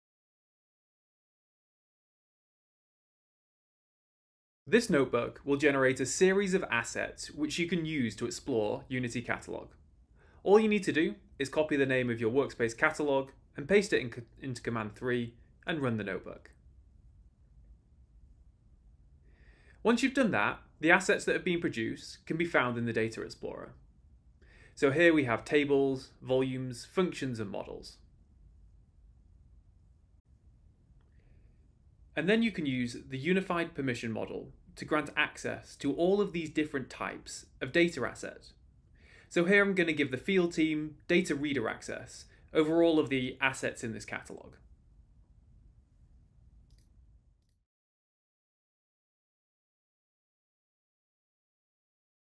Building an Enterprise Data & AI Catalog with Databricks Unity Catalog
Democratizing data and AI requires ensuring the accessibility and security of data and AI assets. Databricks Unity Catalog simplifies this process by providing users with a central hub for effortlessly managing their data and AI resources. This empowers users to generate tables, AI models, functions, and files and catalog them in one place while maintaining access controls and auditability